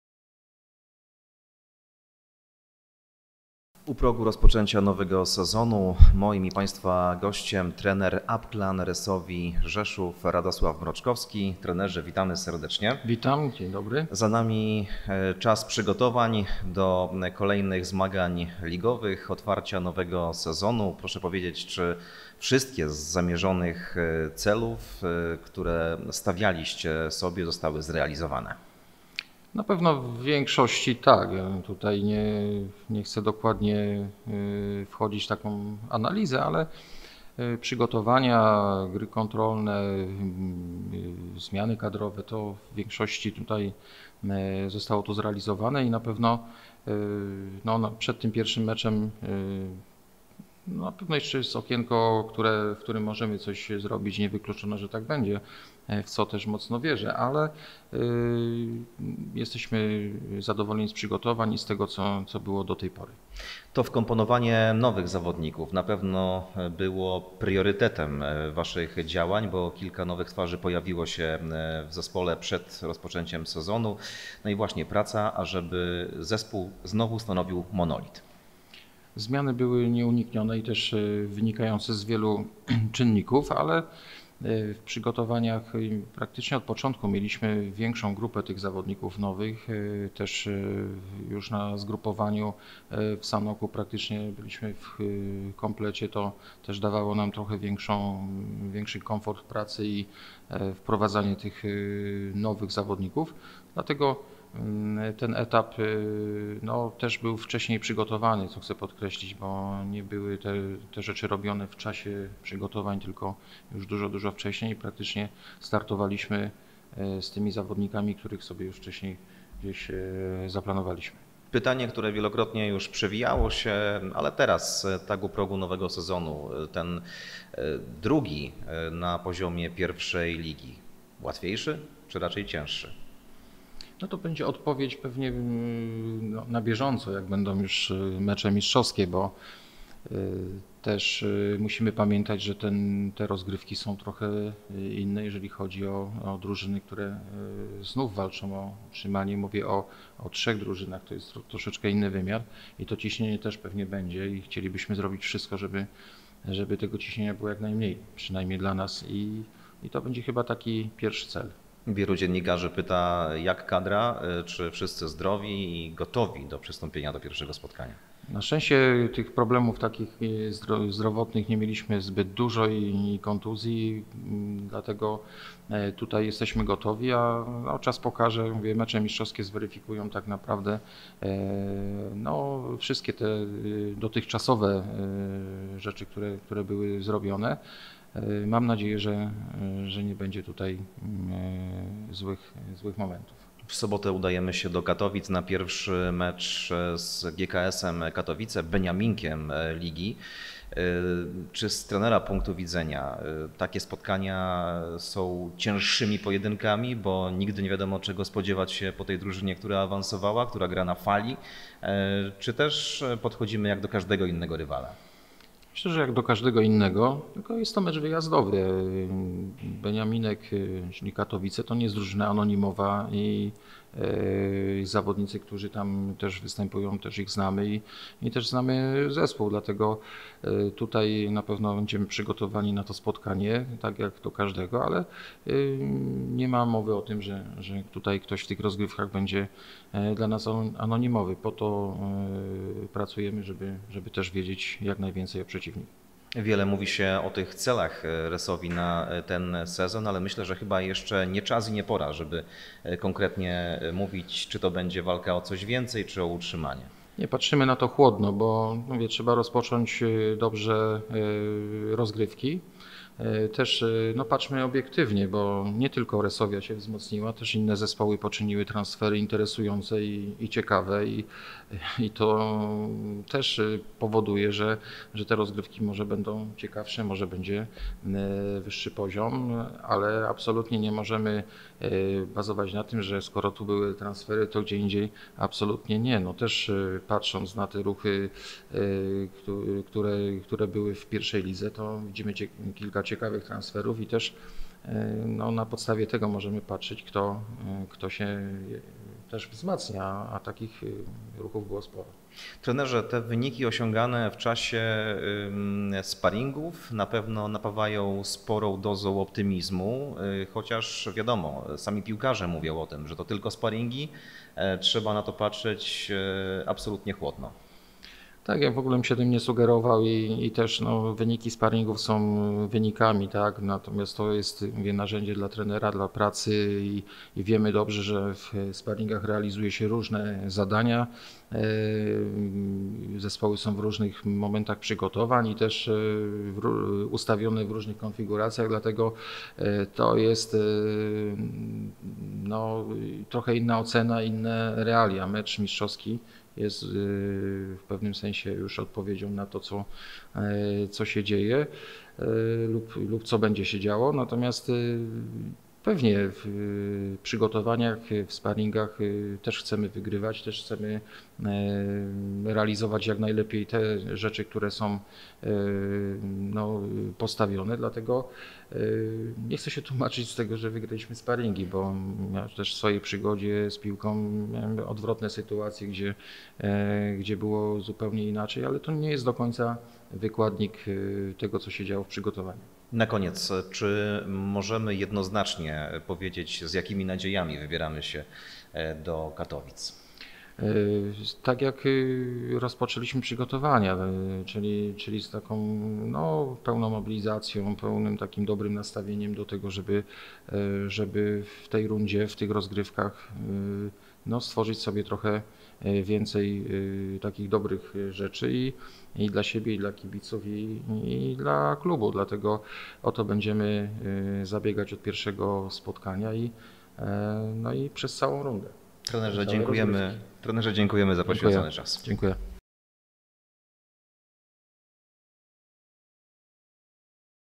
PRZED MECZEM Z GKS KATOWICE | RADOSŁAW MROCZKOWSKI
W najbliższą sobotę inaugurujemy rozgrywki Fortuna 1 Ligi, gdzie w meczu 1. kolejki zmierzymy się z GKS Katowice. Na dwa dni przed meczem porozmawialiśmy z trenerem Radosławem Mroczkowskim. Zapraszamy do zapoznania się z materiałem wideo.